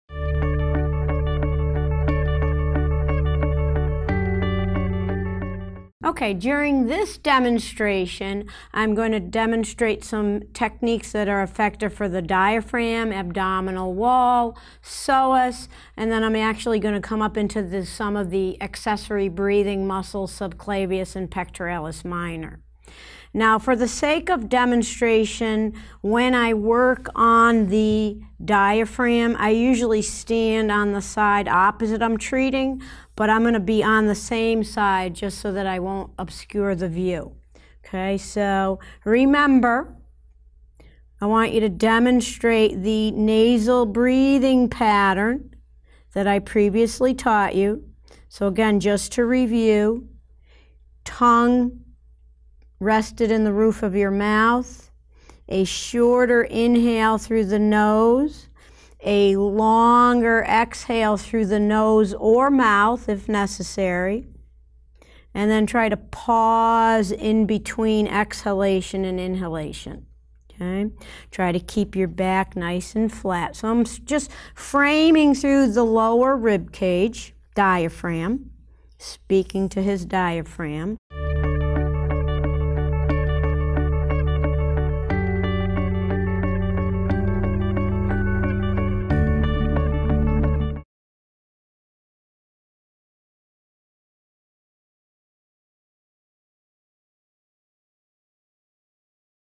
Applying Graston Technique® Therapy to the Diaphragm - Jacqueline Shakar | MedBridge
Read related article: "Are Your Patients Breathing or B-R-E-A-T-H-I-N-G?"